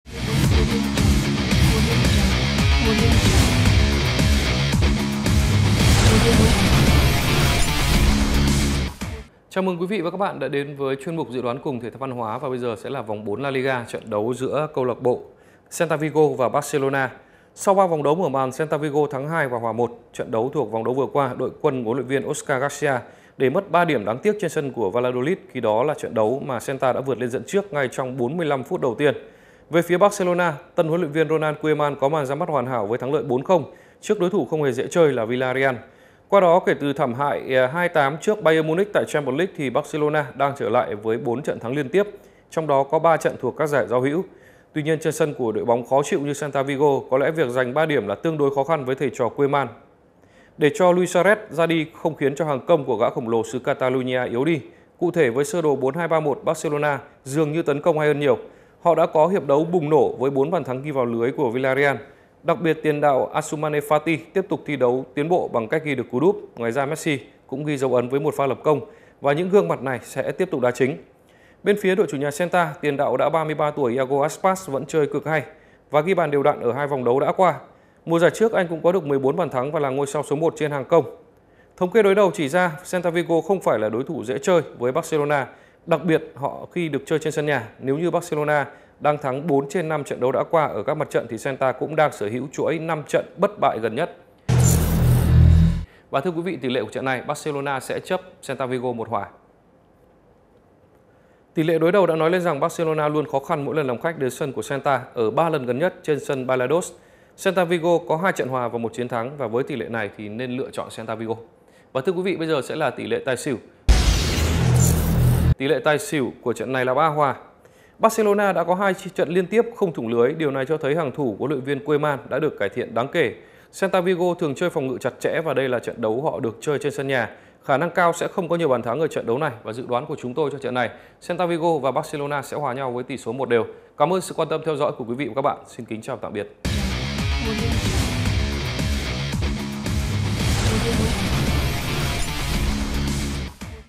[NHẬN ĐỊNH BÓNG ĐÁ] Soi kèo Celta Vigo - Barcelona (2h30 ngày 2/10). Vòng 4 La Liga. Trực tiếp BĐTV
Tân HLV Ronald Koeman đã có màn ra mắt hoàn hảo cùng CLB Barcelona bằng thắng lợi 4-0 trên sân nhà Nou Camp trước đối thủ không hề dễ chơi chút nào Villarreal. Siêu sao Messi tiếp tục ghi dấu ấn với 1 bàn thắng.
Qua đó, kể từ trận thảm hại 2-8 trước Bayern Munich tại Champions League, Barca đang trở lại với 4 trận thắng liên tiếp. Trong đó có 3 trận thuộc các giải đấu giao hữu.
Tuy nhiên, việc phải thi đấu vòng 4 La Liga Tây Ban Nha trên sân của đội bóng khó chịu như Celta Vigo, có lẽ việc giành 3 điểm là tương đối khó khăn với thày trò Koeman.
* Tỷ lệ/kèo 90 phút: Celta Vigo 1 : 0 Barcelona
 Tỉ lệ đối đầu đã nói lên rằng Barcelona luôn khó khăn mỗi lần làm khách đến sân của Celta, ở 3 lần gần nhất trên sân Balaídos, Celta Vigo có 2 trận hòa và 1 chiến thắng. Với tỉ lệ này thì nên lựa chọn cửa Celta.
* Tài xỉu/Tổng số bàn thắng: 3 hòa
 Barcelona đã có 2 trận liên tiếp không thủng lưới, điều này cho thấy hàng thủ của HLV Koeman đã được cải thiện đáng kể. Celta Vigo thường chơi phòng ngự chặt chẽ và đây là trận đấu họ được chơi trên sân nhà. Khả năng cao sẽ không có nhiều bàn thắng ở trận đấu này.
* Dự đoán của TT&VH: Celta Vigo - Barcelona: 1-1
* Lịch thi đấu và truyền hình trực tiếp vòng 4 La Liga Tây Ban Nha:
0h00 ngày 30/9: Real Sociedad - Valencia (BĐTV)
2h30 ngày 30/9: Getafe - Betis (BĐTV)
0h00 ngày 1/10: Huesca - Atletico Madrid
0h00 ngày 1/10: Villarreal - Alaves (BĐTV)
2h30 ngày 1/10: Eibar - Elche (BĐTV HD)
2h30 ngày 1/10: Real Madrid - Valladolid (BĐTV)
0h00 ngày 2/10: Athletic Bilbao - Cadiz (BĐTV)
0h00 ngày 2/10: Sevilla - Levante (BĐTV HD)
2h30 ngày 2/10: Celta Vigo - Barcelona (BĐTV)
2h30 ngày 2/10: Granada - Osasuna (BĐTV HD)